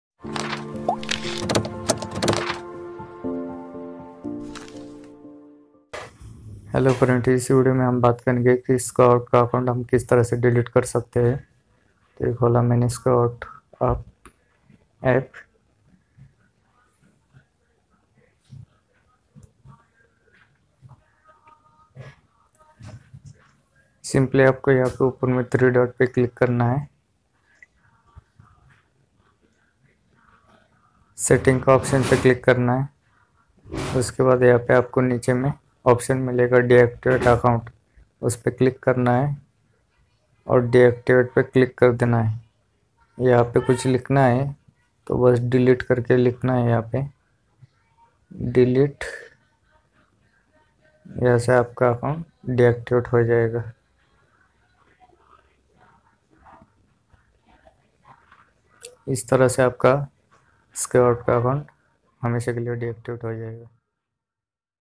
Skout Account Delete | how to delete skout account | Skout blocked my account
Skout Account Delete | how to delete skout account | Skout Account delete kaise karen | skout delete

Hello friends Is video Main Hum Baat karange Aap Aapna Account Permanently Delete Ya Temporary Delete Kise Karenge